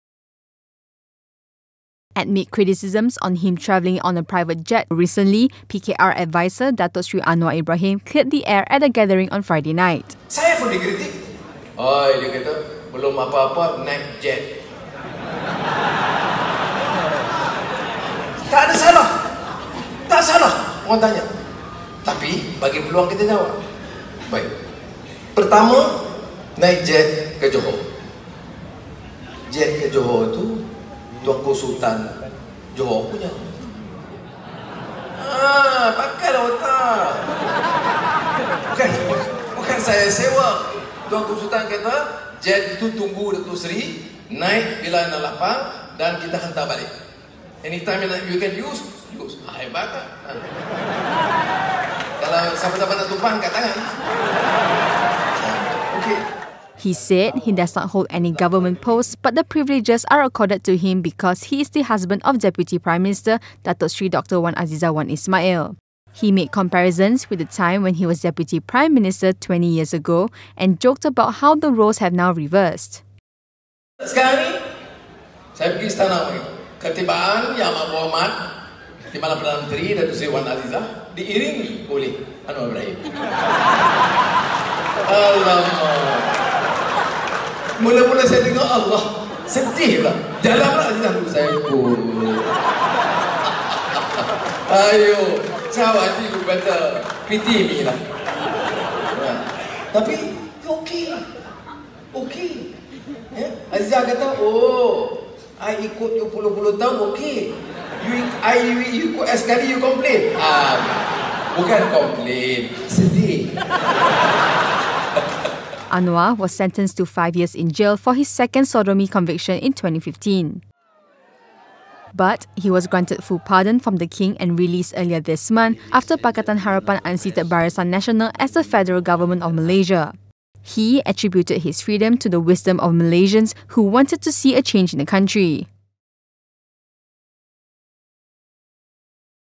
Anwar explains private jet and jokes about role reversal
PKR adviser Datuk Seri Anwar Ibrahim said the private jet he and his family used for travelling to Johor recently belongs to the Sultan of Johor. At a gathering in Penang on Friday night, he also joked role reversal between him and his wife.